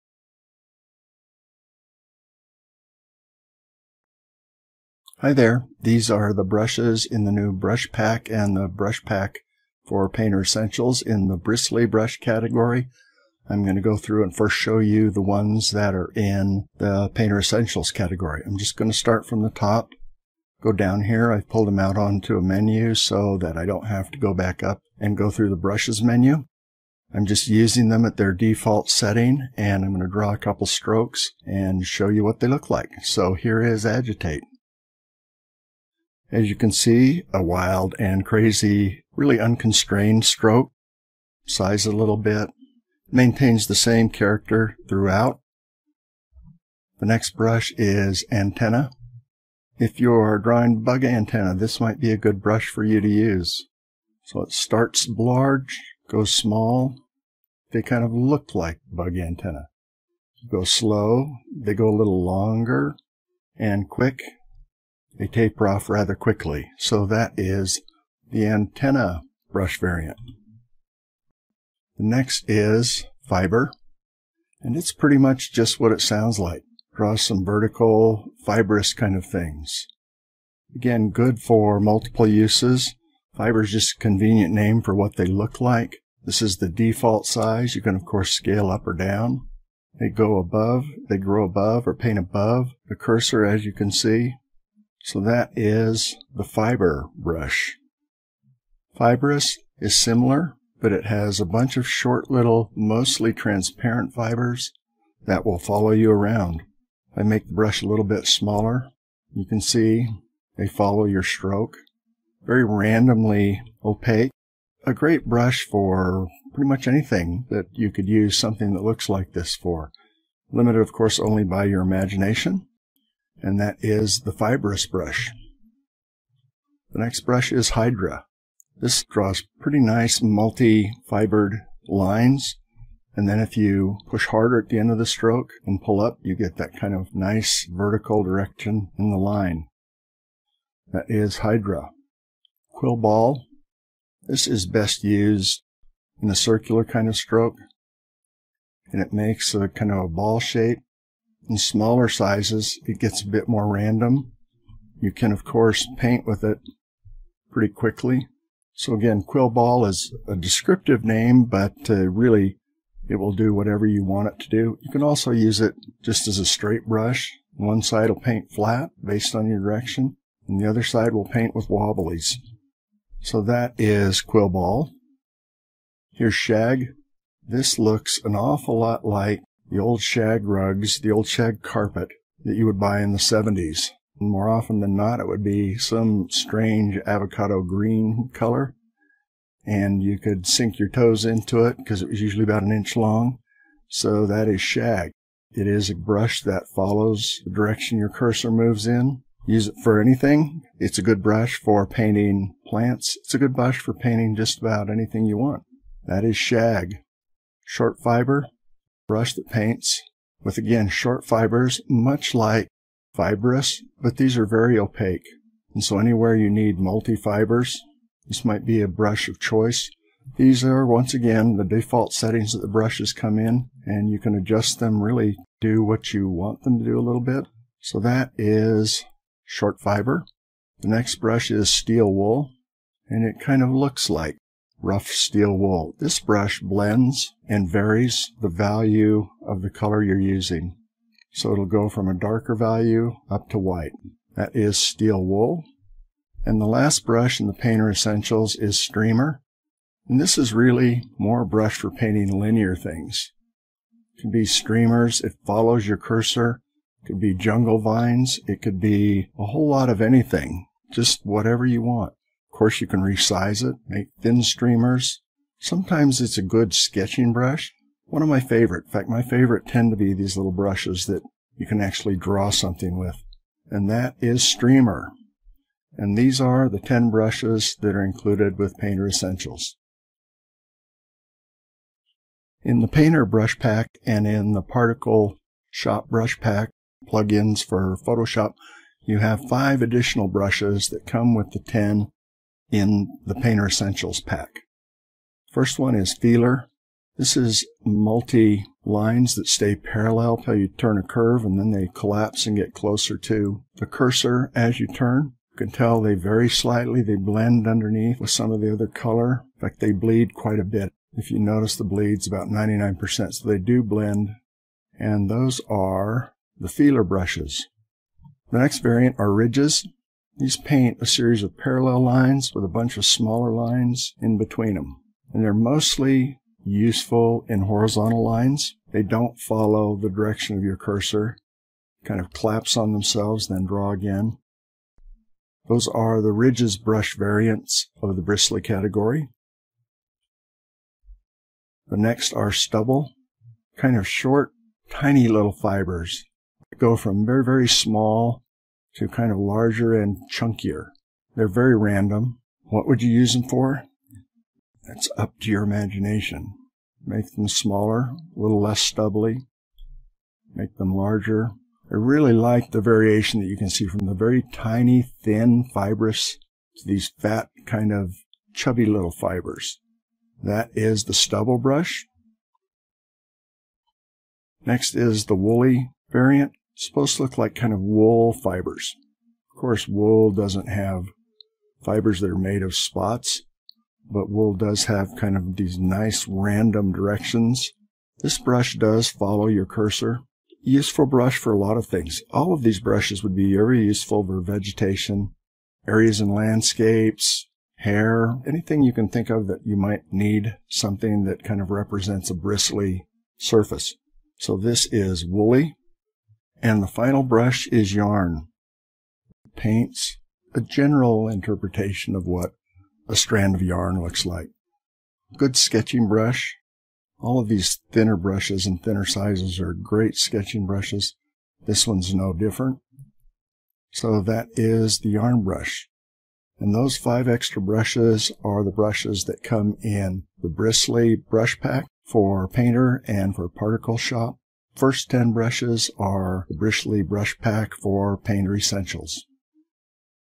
How to use the Bristly Brush Pack for Painter and ParticleShop
Imagine shag carpet where you can dig in and wiggle your toes. Or marvel at an insect’s long antenna, and enjoy the fun of unwinding a ball of yarn. Bring these kinds of materials to life using the Bristly Brush Pack. From yarn to steel wool, it can all be yours. The only limitation to using this wide variety of brushes is your imagination.